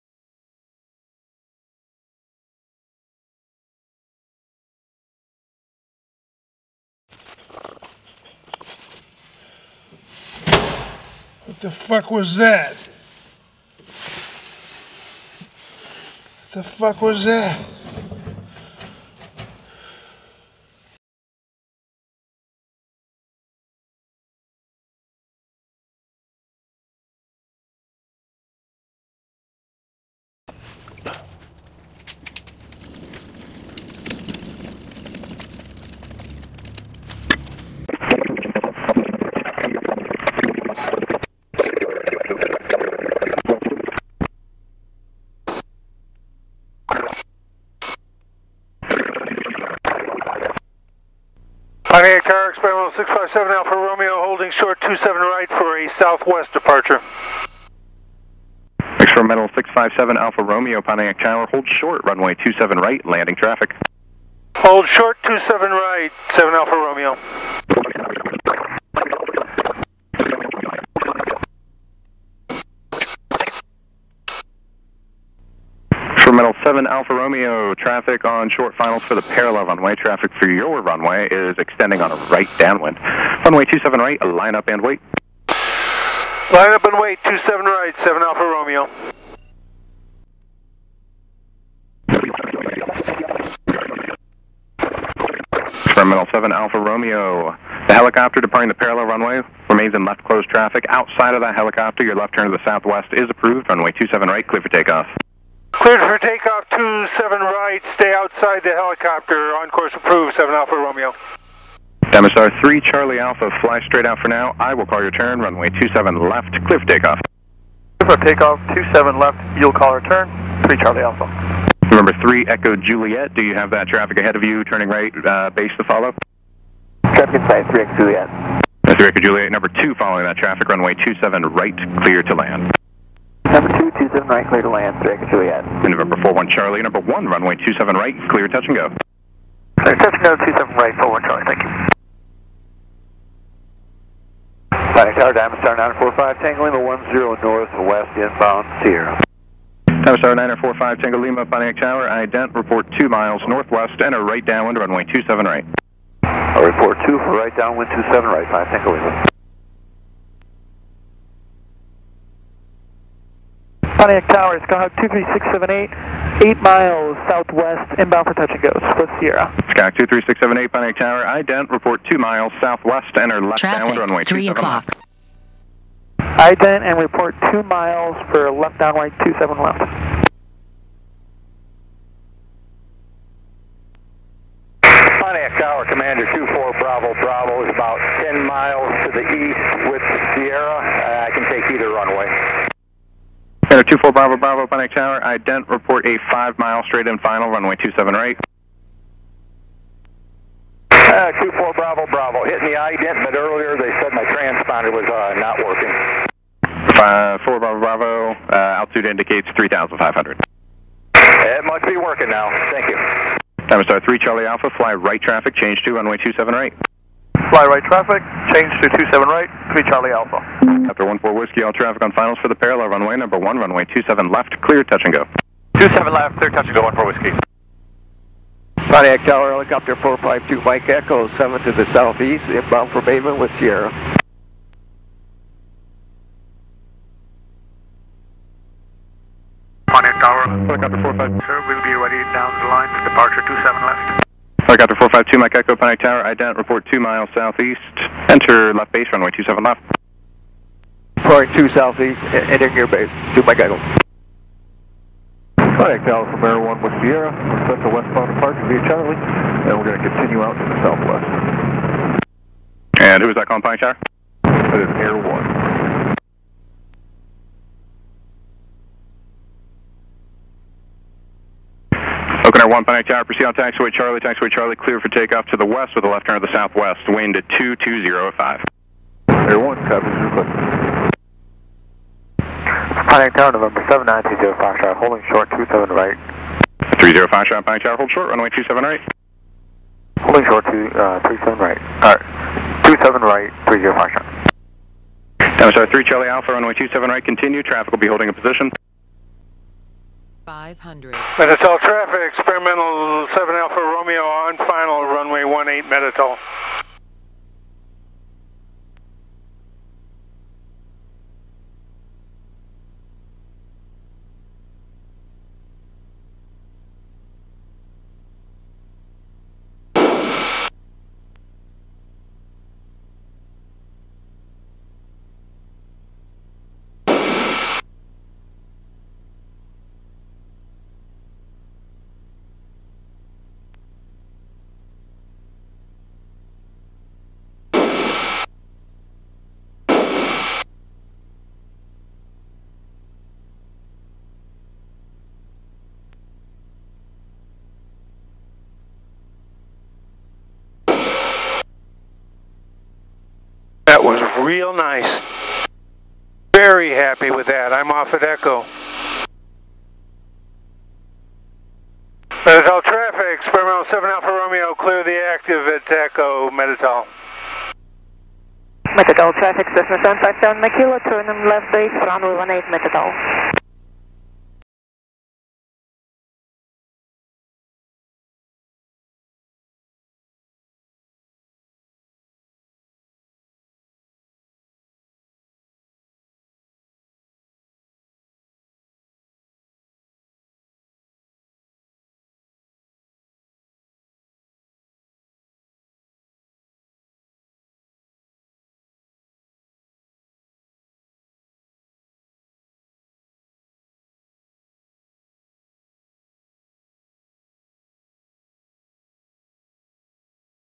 Chest Camera Mount Test Flight 221201
WARNING: EXPLICIT CONTENT.  This one is crude and rude; it's for my test archives but I'll share it anyway.  Couldn't resist the sarcastic text for my own amusement.  No music; mercifully short.  It was fun to finally fly to a EAA Chapter 113 homebuilder's meeting, get some night flight in and conduct tests all in the same evening.